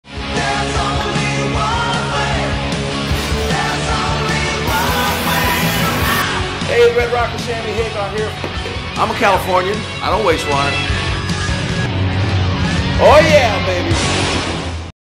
Don't Waste Water: Sammy Hagar
He's an R-N-R Hall of Famer. A California boy, born and bred. And he's well aware of the drought that's hit his home state.